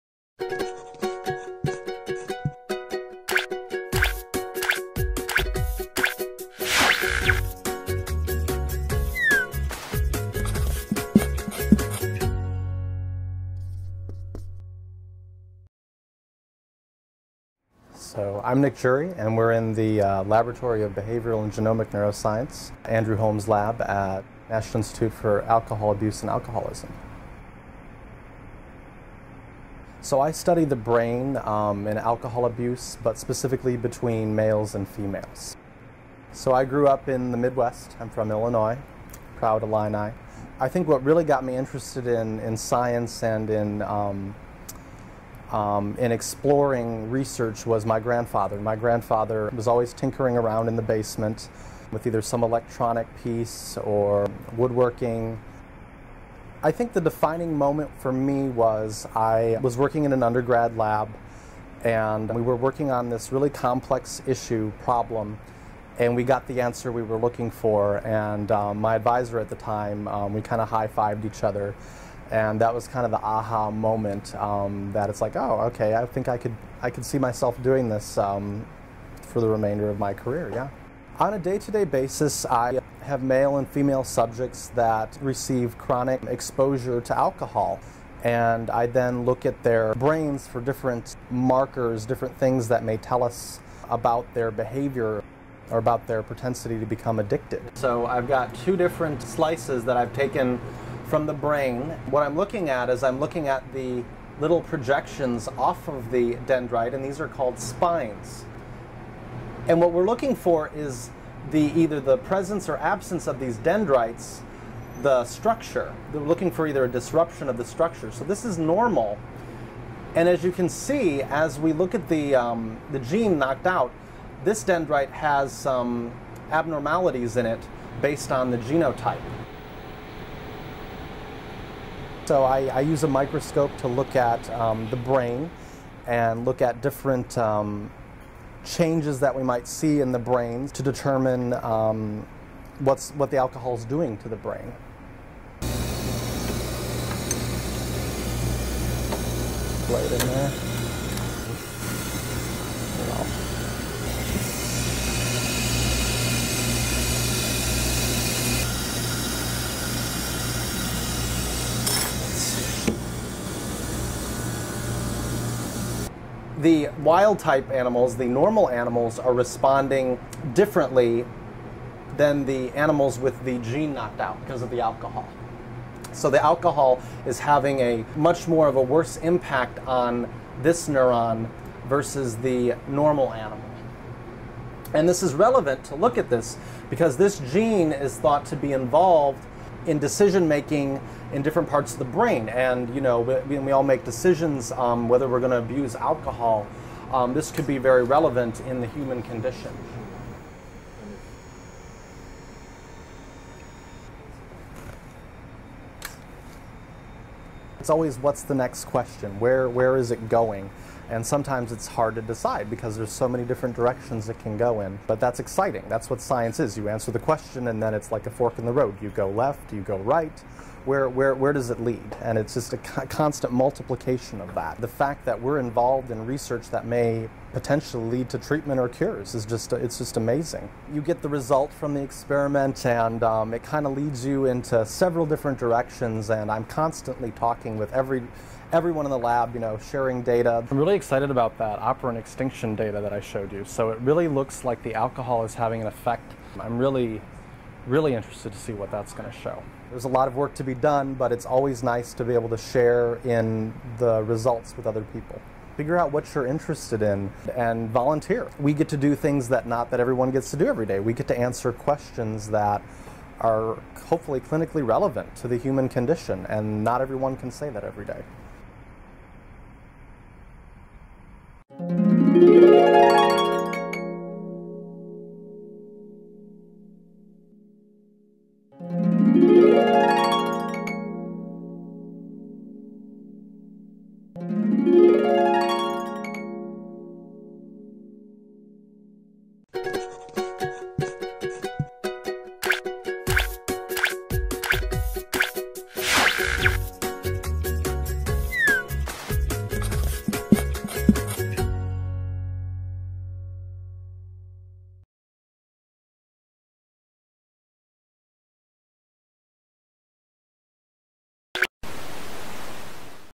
NIAAA: Postdoctoral Alcohol Abuse Research
Nick Jury is a postdoctoral fellow in the National Institute on Alcohol Abuse and Alcoholism (NIAAA) Laboratory of Behavioral & Genomic Neuroscience. Watch this video to learn more about how Dr. Jury is looking at the differences in alcohol abuse between males and females.

For more information about the National Institute on Alcohol Abuse and Alcoholism, visit niaaa.nih.gov.